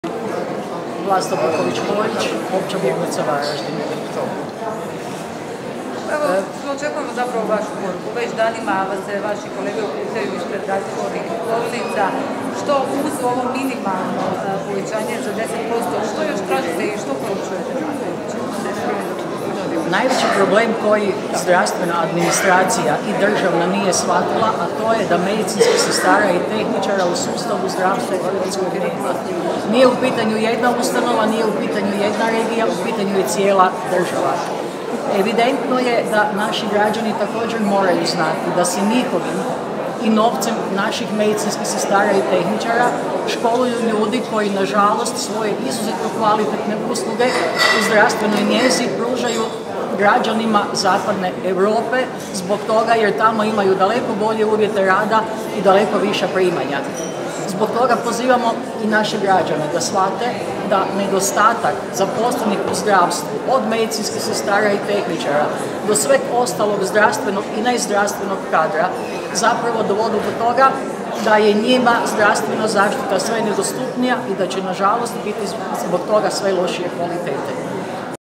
Vlasta Vuković Hoić: "Građanima je zdravstvena zaštita sve nedostupnija"
Nastavno na prosvjedne skupove ispred zdravstvenih ustanova, Hrvatski strukovni sindikat medicinskih sestara - medicinskih tehničara i Samostalni sindikat zdravstva i socijalne skrbi Hrvatske održao je konferenciju za medije u Maloj dvorani KD Vatroslav Lisinski.